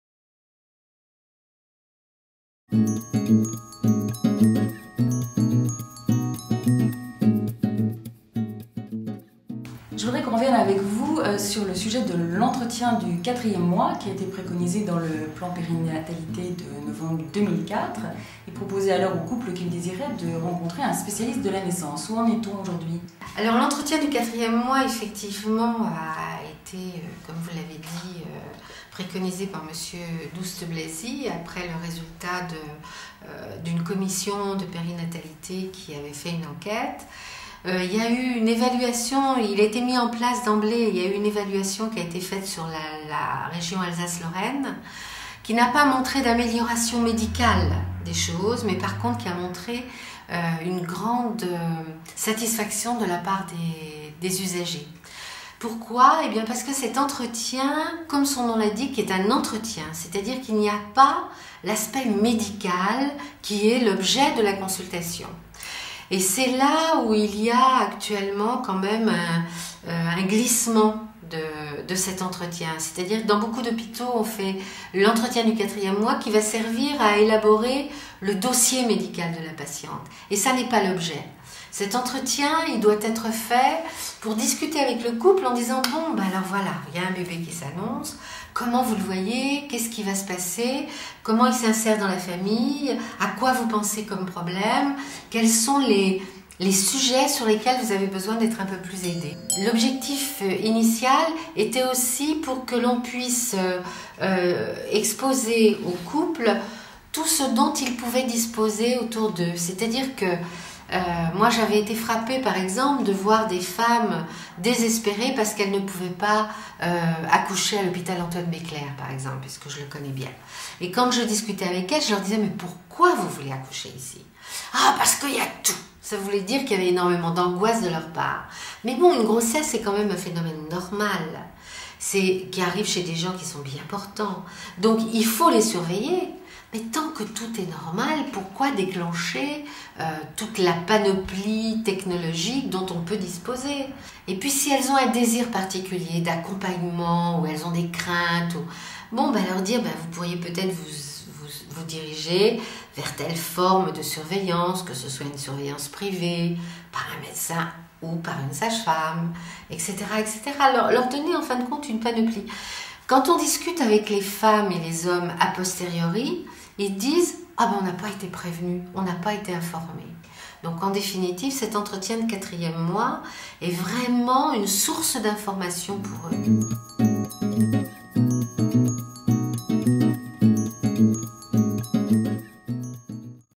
Grossesse : zoom sur l'entretien du 4e mois
Préconisé depuis2 004, cet entretien, qui s'ajoute aux 7visites prénatales, permet aux futurs parents de rencontrer un spécialiste de la naissance. Appelé officiellement entretien individuel du 4e mois, il est systématiquement proposé à la femme enceinte ou au couple. Contrairement aux autres consultations, il n'est pas obligatoire, mais conseillé. Francine Dauphin, sage-femme, nous en dit plus.